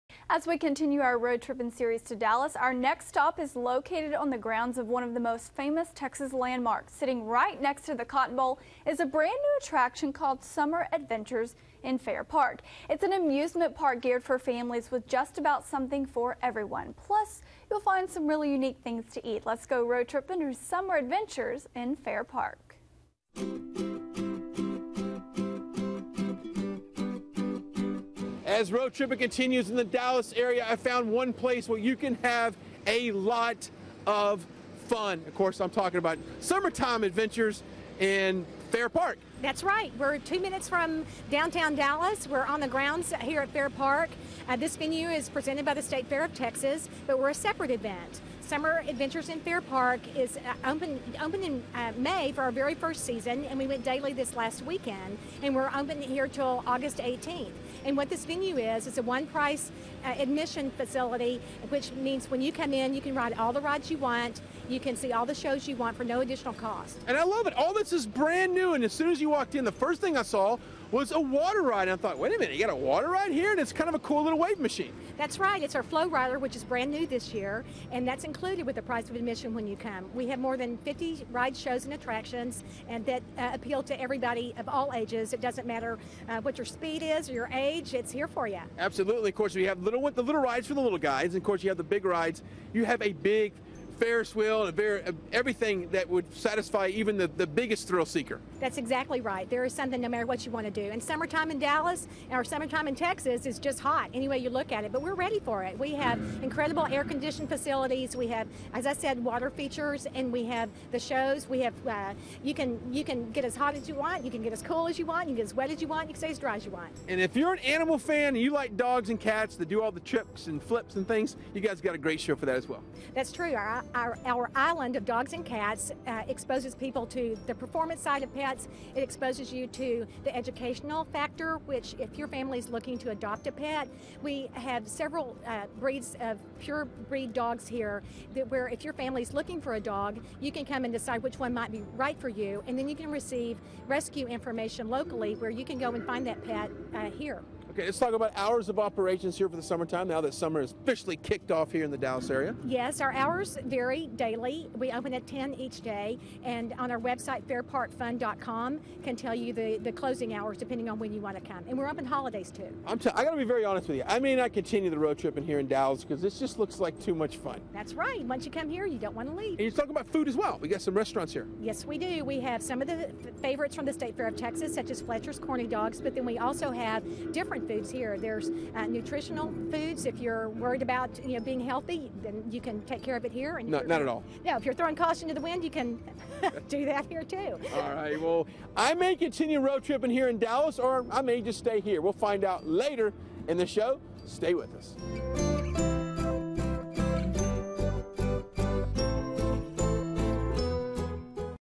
Road Trippin' Dallas: Summer Adventures
It's located on the grounds of one of the most famous Texas landmarks. Sitting right next to the Cotton Bowl is a brand new attraction called Summer Adventures in Fair Park.